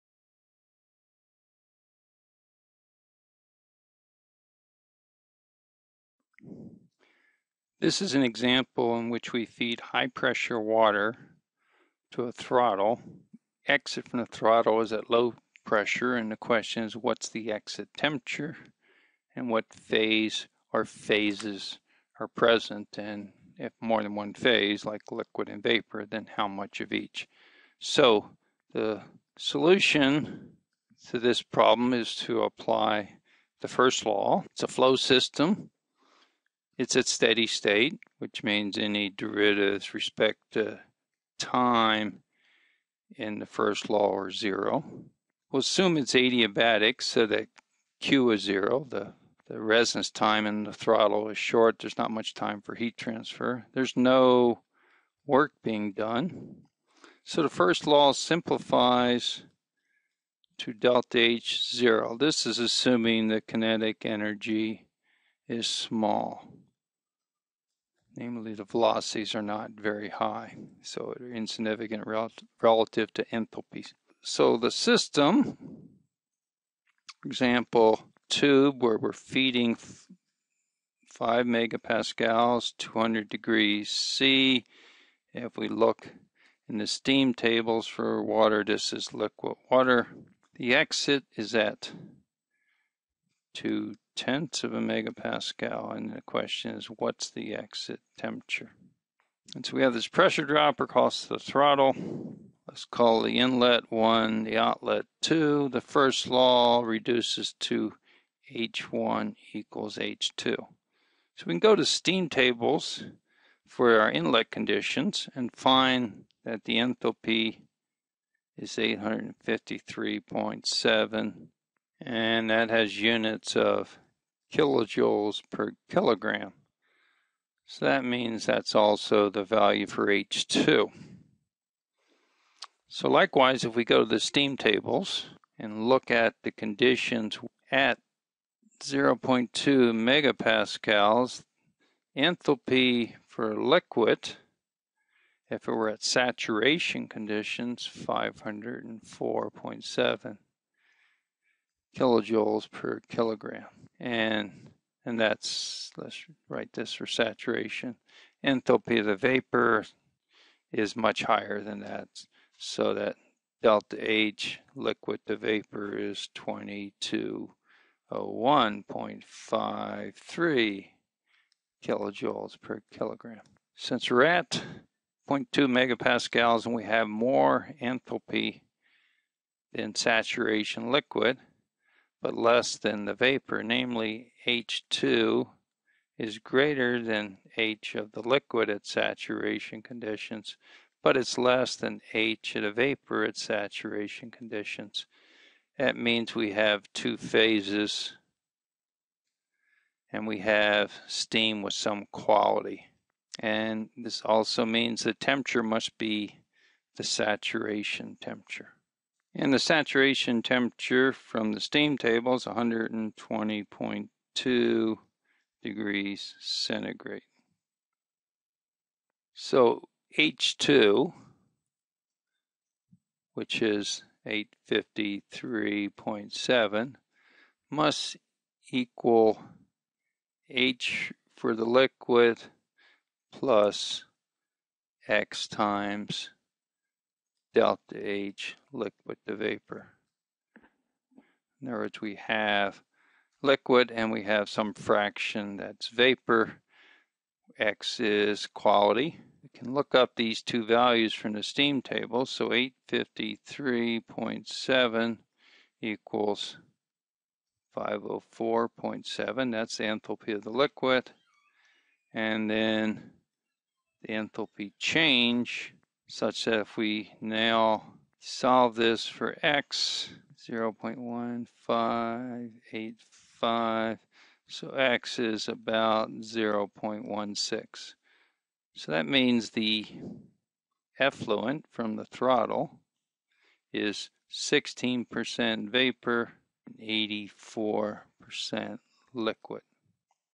Throttle Example: High-Pressure Liquid
Use steam tables to determine the outlet temperature when a high-pressure liquid is throttled to lower pressure. Made by faculty at the University of Colorado Boulder Department of Chemical and Biological Engineering.